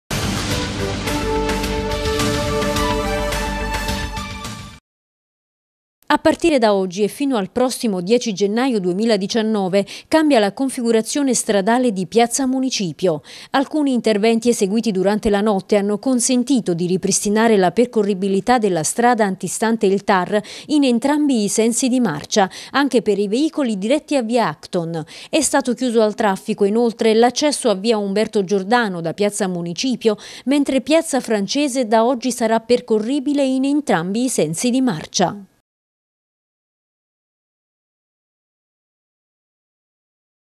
Traffico, nuova viabilità in Piazza Municipio
Ripristinata la viabilità antistante il Tar in entrambi i sensi di marcia. Da questa mattina, e fino a gennaio 2019, sarà chiuso al traffico l'accesso a via Umberto Giordano da Piazza Municipio, mentre piazza Francese sarà percorribile a doppio senso.